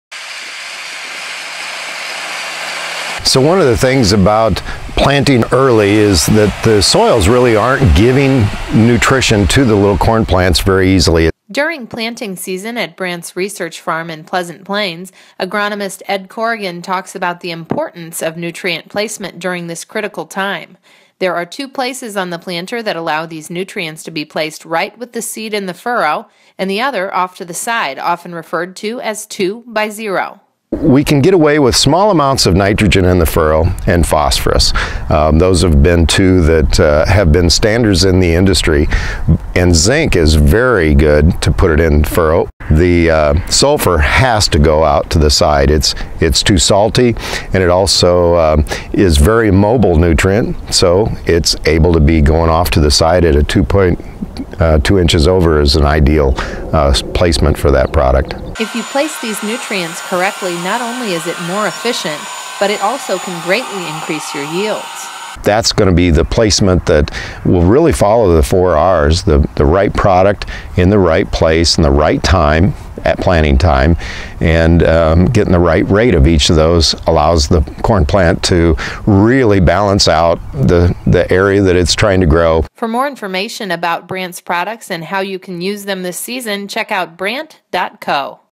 BRANDT Agronomy Update: Nutrition at Planting
BRANDT's Technical Agronomist, Ed Corrigan, talks about getting the right nutrition and the proper amount to your corn plant at planting. Learn how placing these nutrients in the right way can help increase your plant's health and yield.